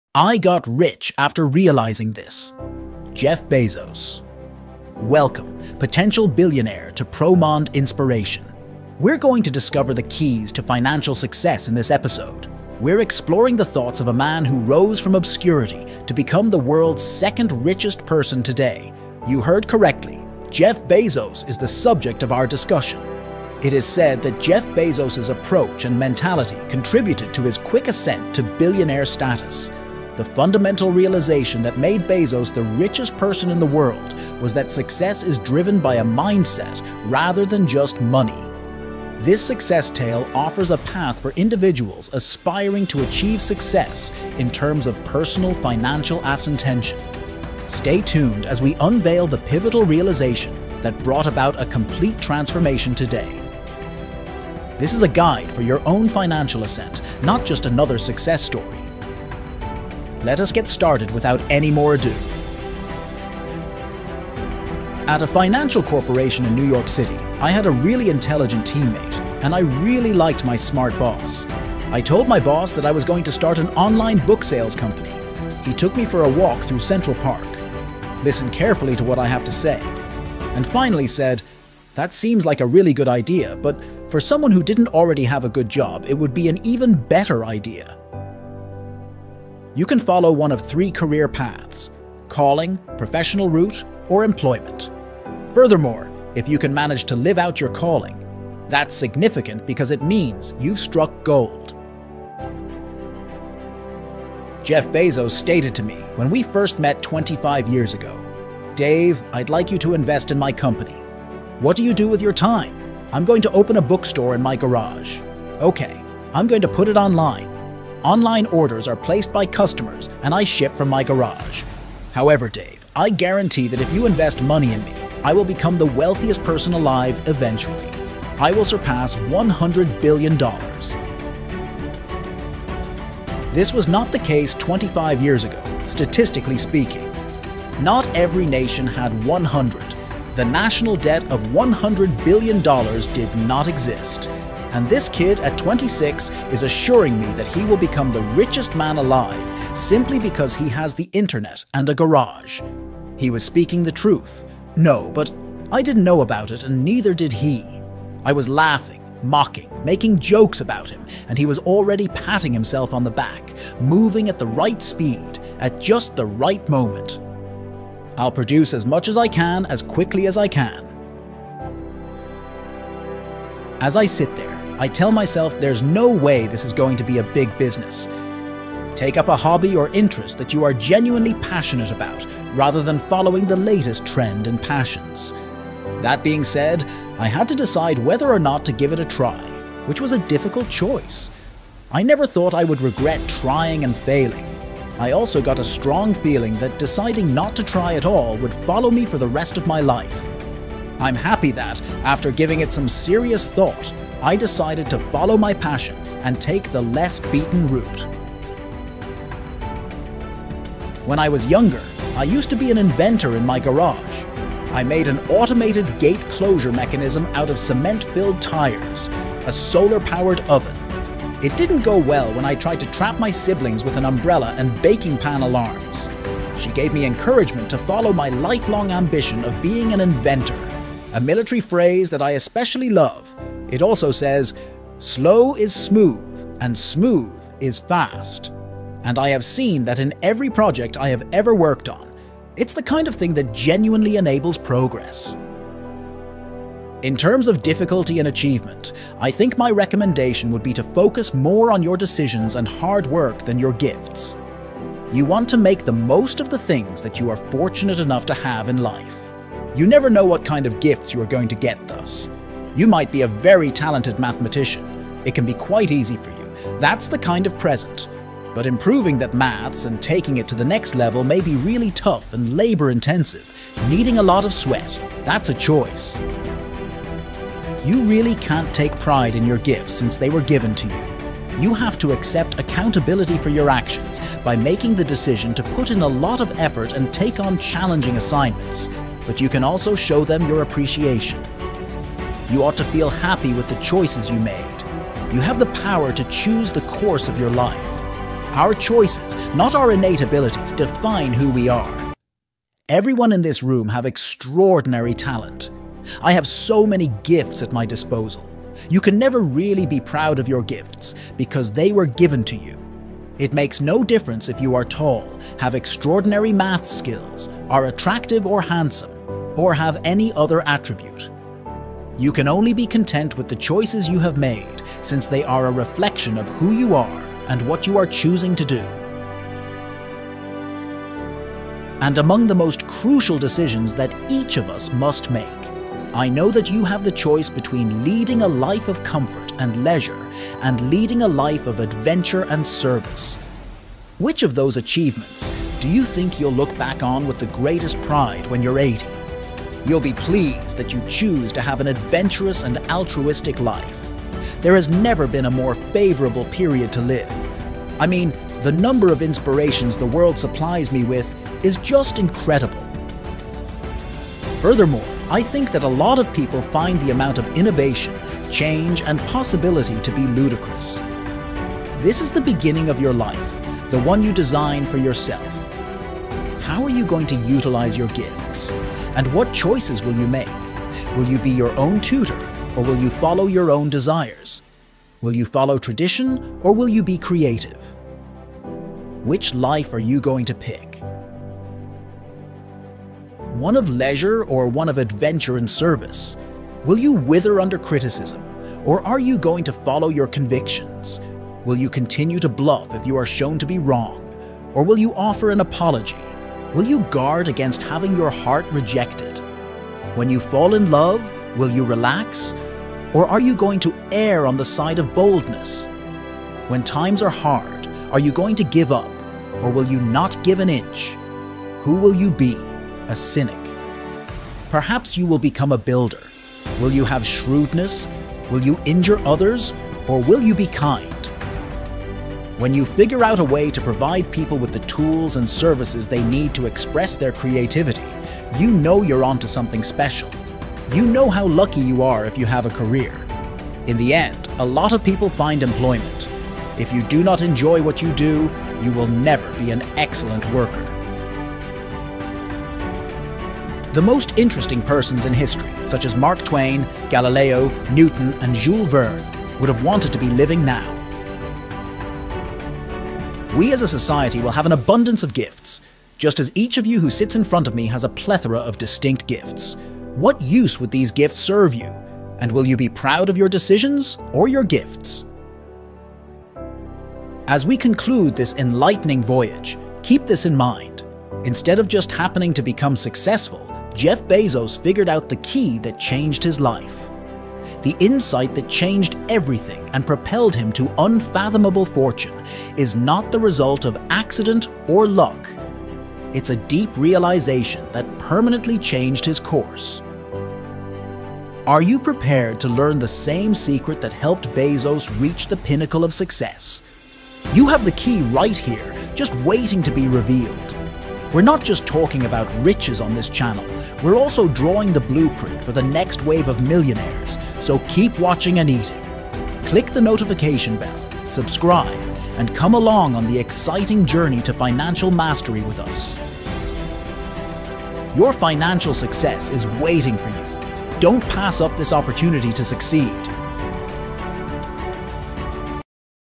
I GOT WEALTHY AFTER REALISING THIS — JEFF BEZOS | Motivation
"I got wealthy after realising that the key to success is not just in getting rich but in understanding how to create value that transforms lives." Jeff Bezos

Unlock the secrets to wealth with Jeff Bezos in this powerful motivational speech. Discover the mindset that propelled him to success and learn how to apply it to your own life. Jeff Bezos shares his journey, business insights, and the key lessons that transformed him into one of the richest entrepreneurs of our time. Don't miss out on the motivation you need to achieve your financial goals! 💼💰